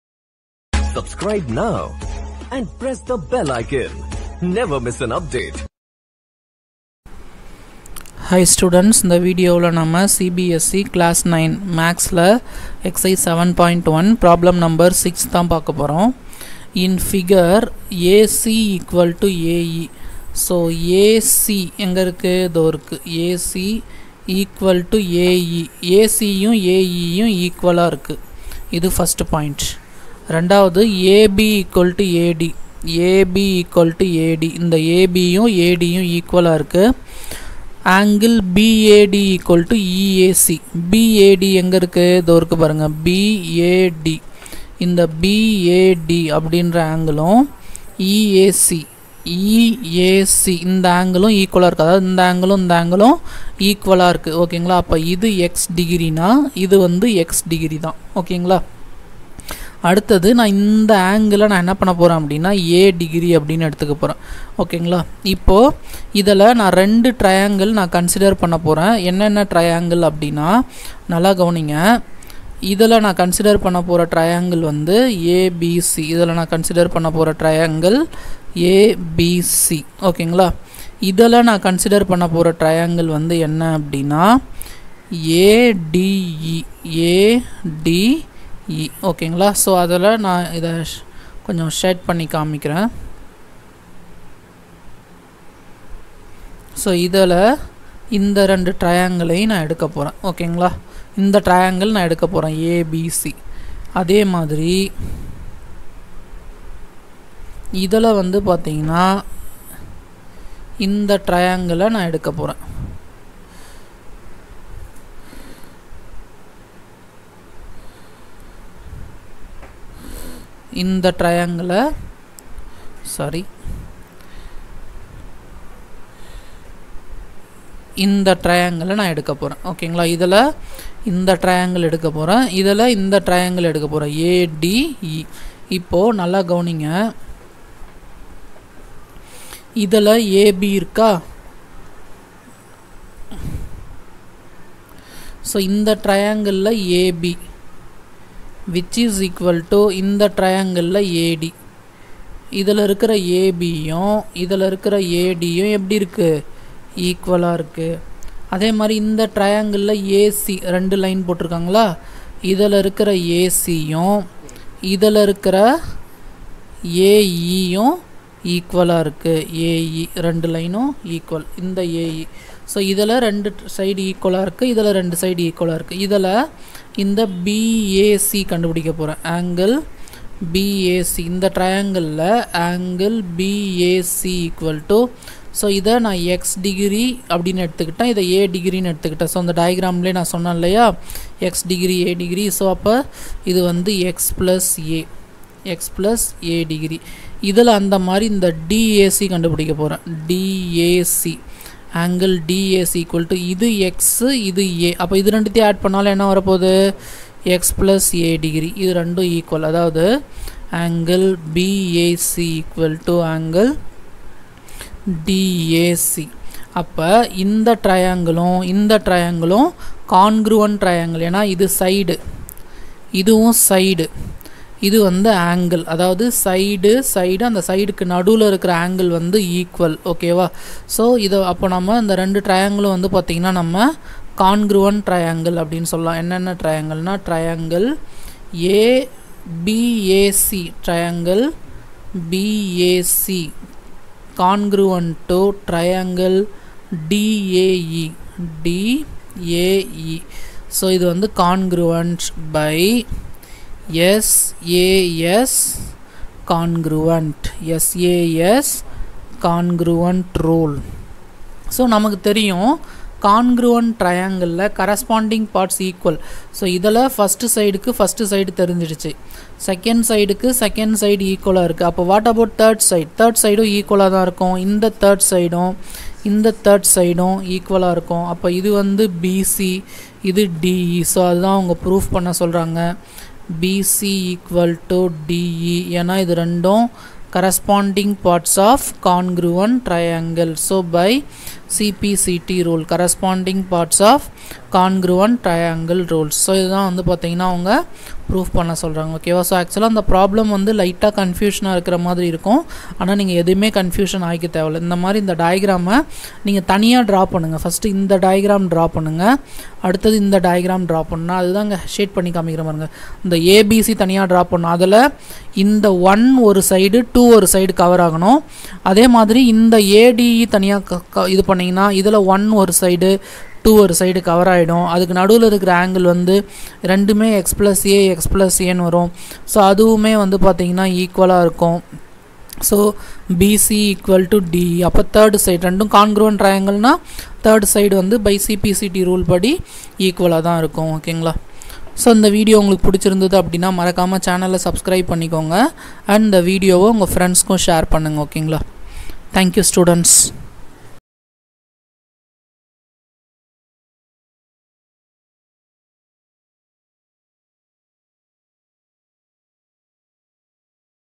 Class 9, Maths Exercise 7.1 Q.No 6 in Tamil | Chapter 7 Triangles | CBSE Maths, NCERT in Tamil
Topics:
7.1 Introduction
7.2 Congruence of Triangles
7.3 Criteria for Congruence of Triangles
7.4 Some Properties of a Triangle
7.5 Some More Criteria for Congruence of Triangles
7.6 Inequalities in a Triangle